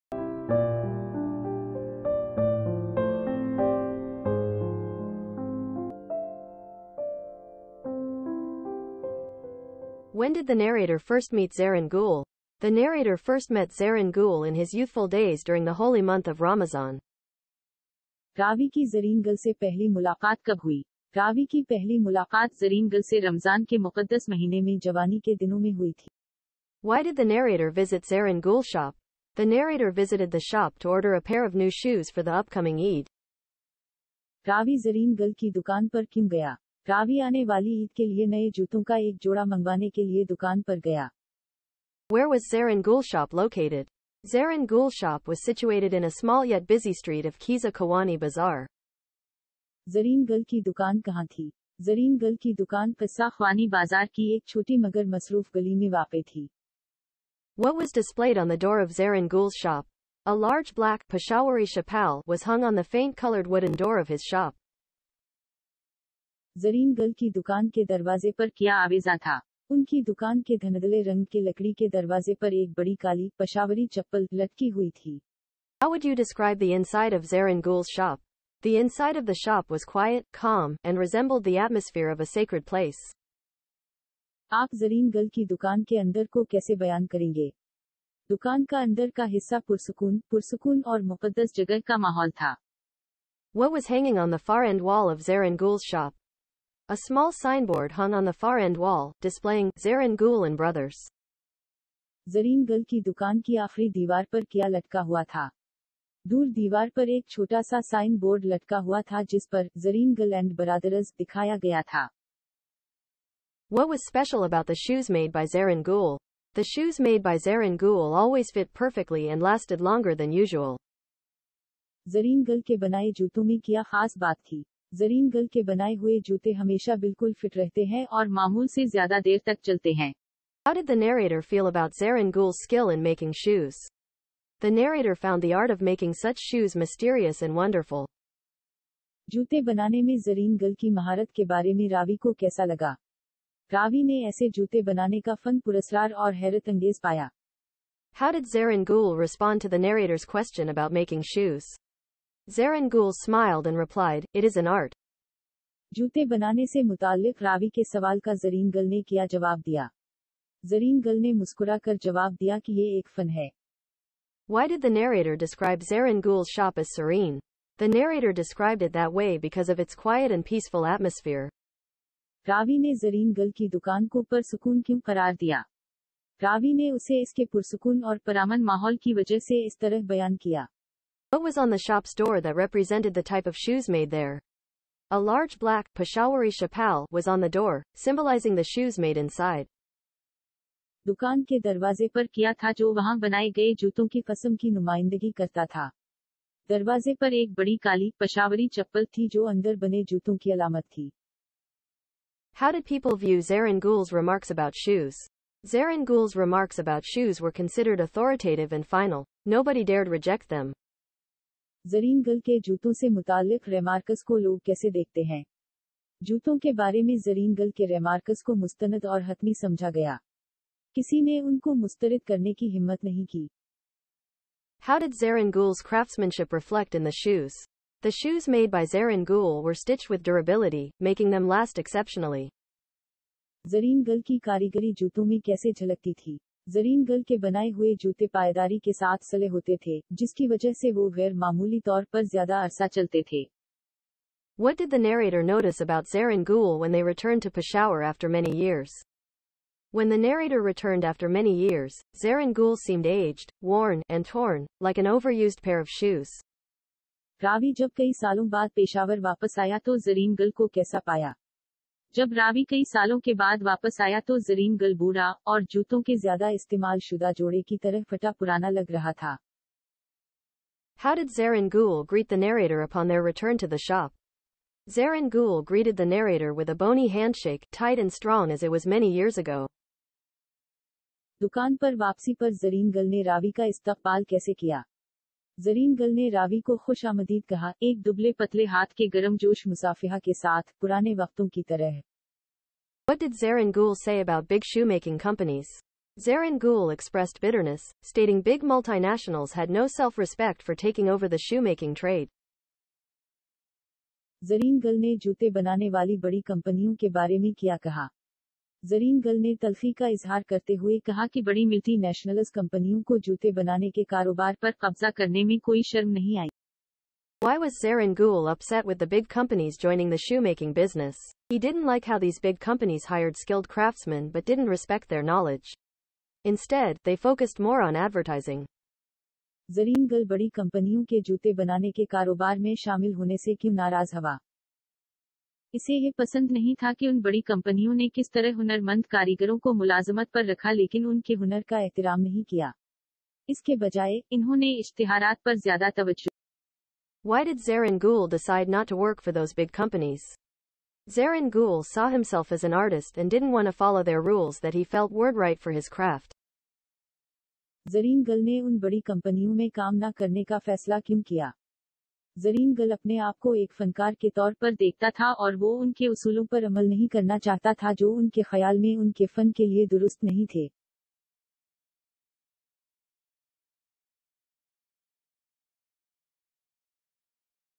SLO Q/A with Urdu Explanation of The Fantastic Shoemaker, Chapter 9, Class 9, Kpk Board
Slo question answers with urdu explanation